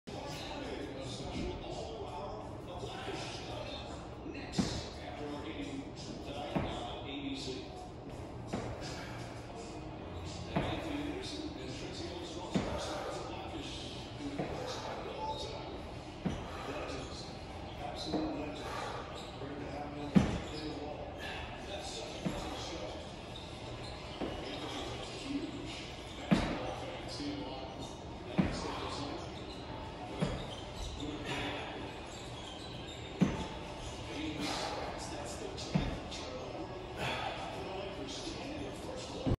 Omnidirectional Arm Driver Lunge
Yeah. I'm sure you care about omnidirectional training... If you do, this is a great warm up